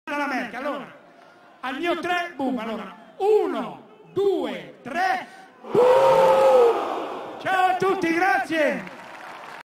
Tsunami Tour Roma 2013 piazza San Giovanni Grillo manda IL BOOM alla Merkel
reference video: "Youmedia"